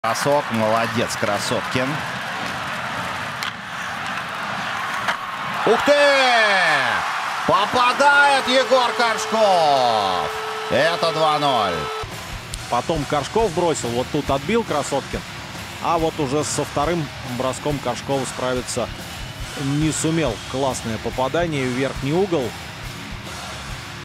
ГОЛ Егор Коршков Амур v Сибирь 4-0 КХЛ 2023-24#shorts
ГОЛ. Егор Коршков. Амур v Сибирь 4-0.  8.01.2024